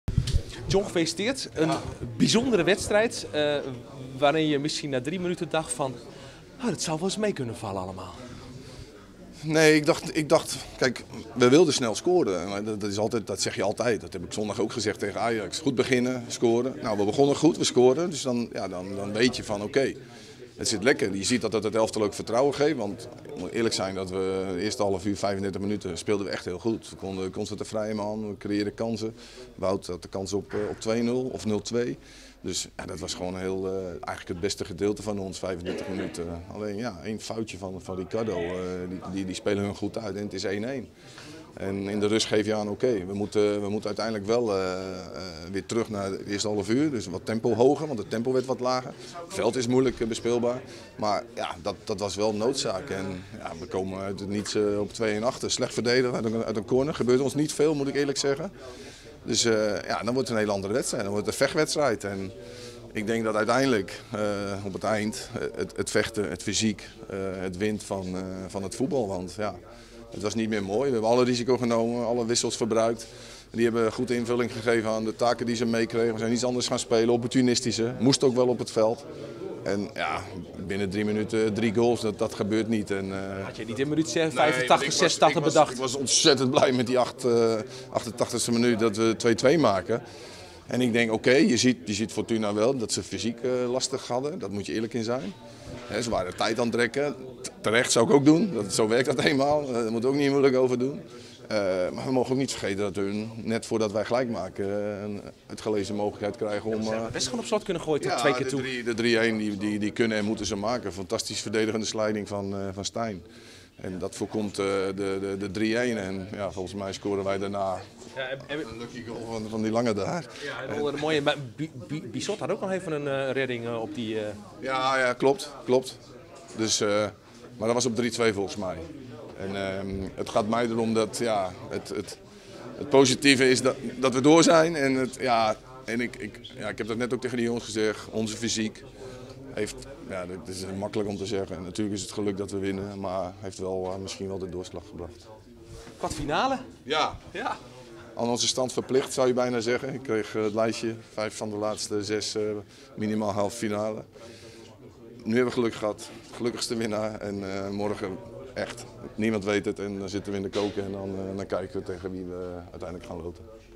Reactie Van den Brom na Fortuna - AZ
John van den Brom blikt met AZ TV terug op de bekerzege in Sittard (2-4).